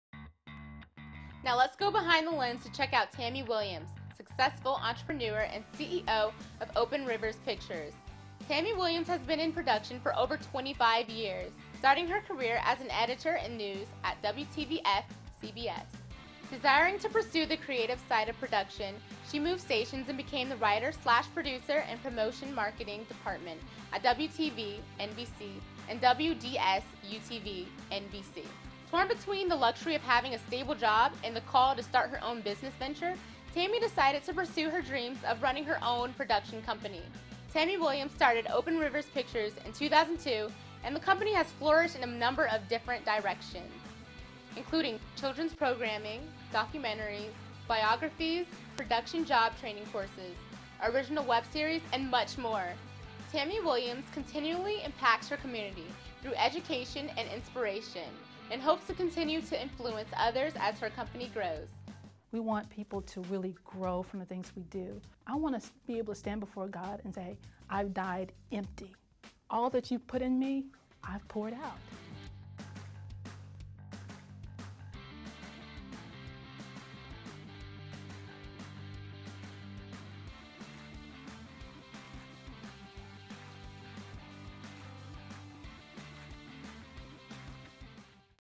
FanFare| Tammy Williams CEO of Open Rivers Pictures
FanFare Host Kali introduces Tammy Williams who is the CEO/Founder of:  Open Rivers Pictures, Open Rivers Film Academy and the creation of FanFare The P.A. Project.